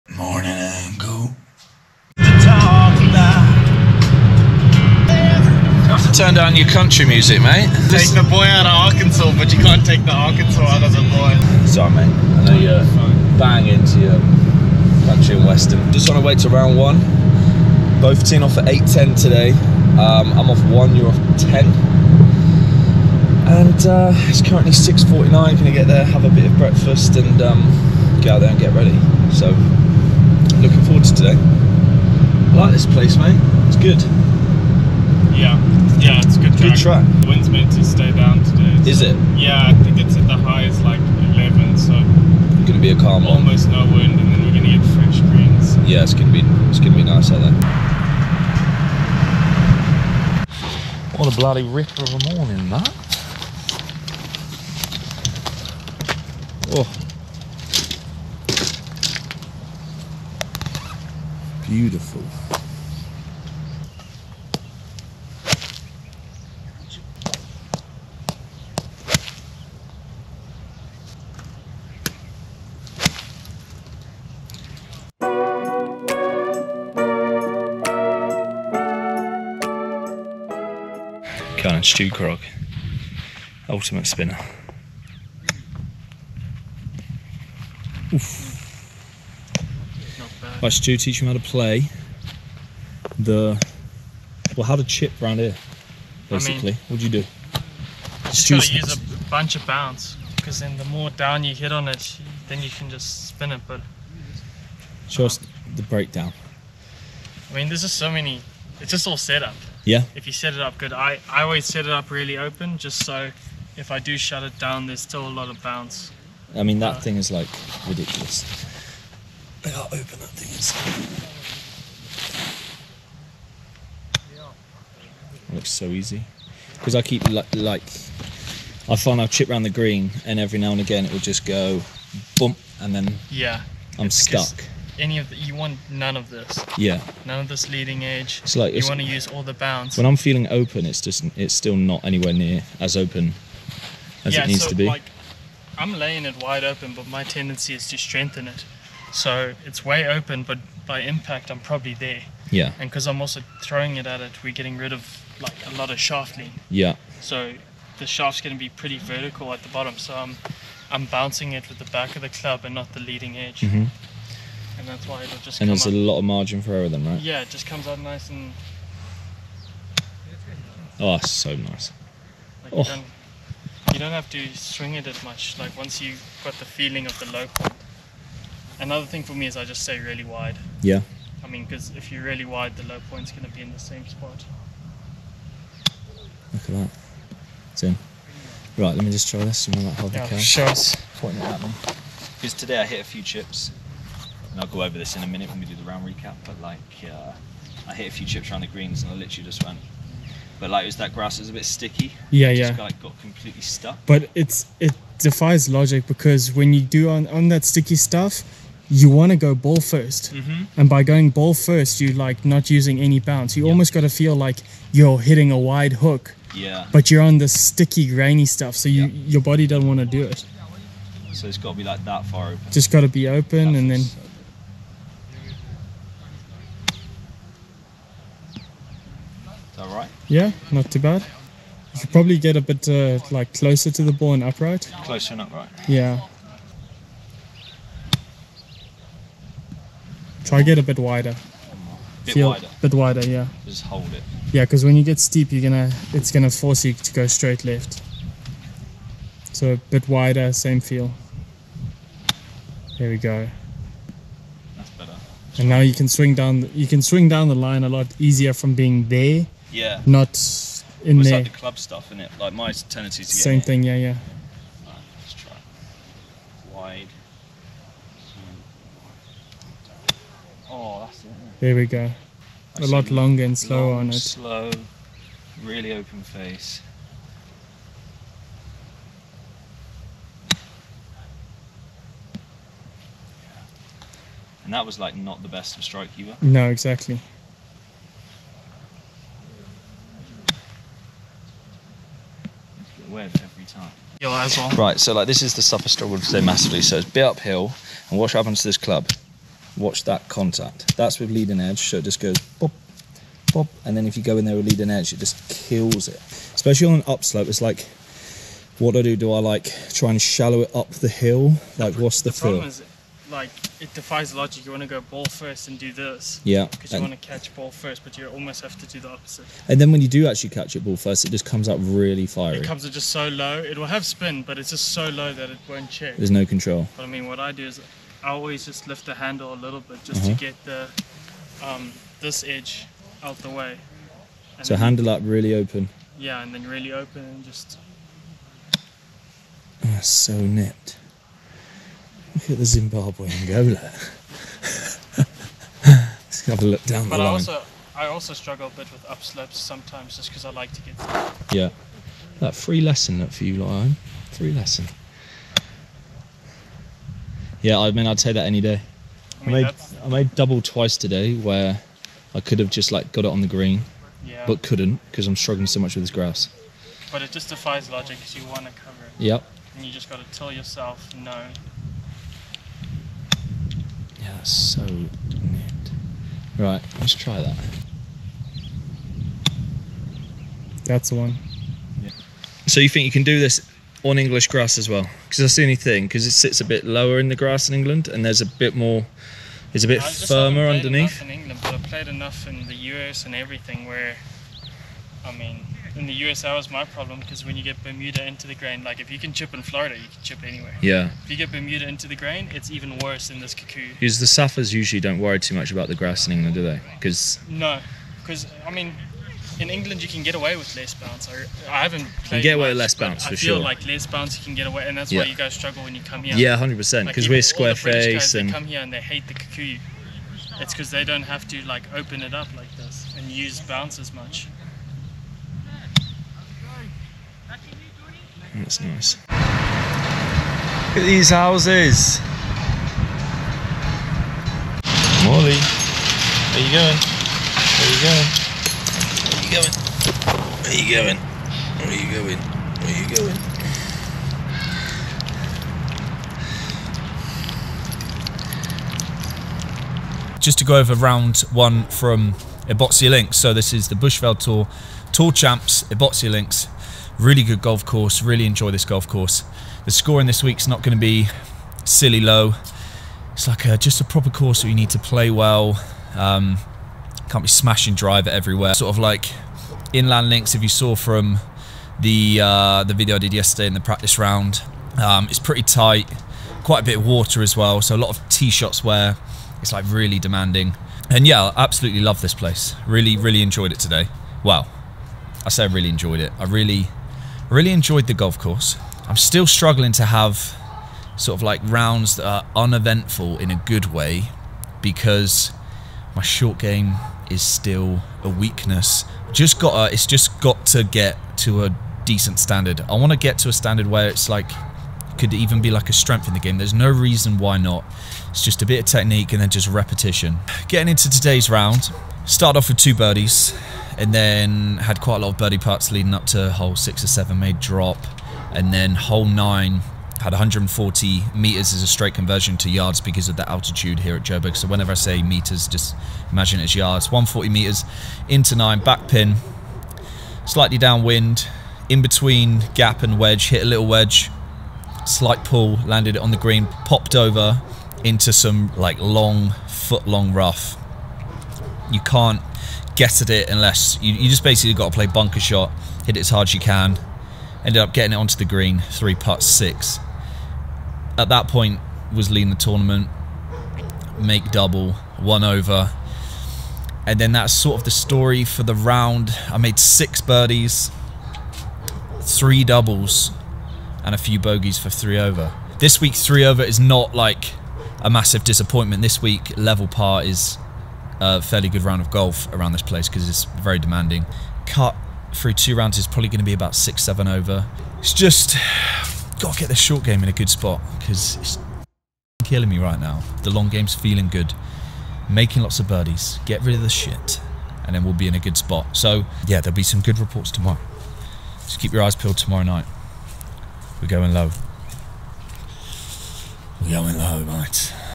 Round 1 Recap + Short Game Lesson...
Round 1 from the Bushveld Tour this week at Ebotse Links Golf Club + A short game lesson from Stu Krog